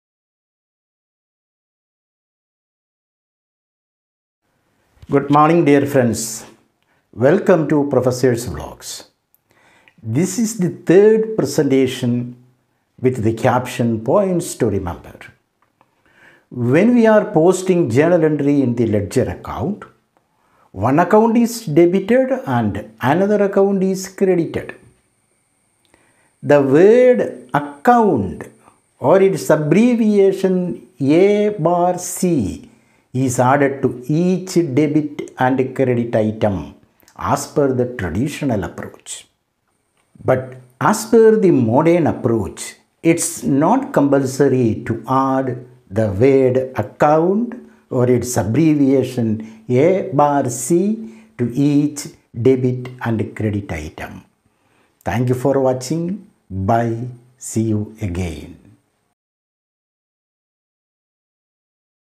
Prefixes in Accounts
In this Master Class Dr. Sunnykutty Thomas of Professors Vlogs discusses the modern approach of prefixing in accounts
Professors Vlogs is a destination for accounting tips and lessons that teaches you how to improve your accounting skills and knowledge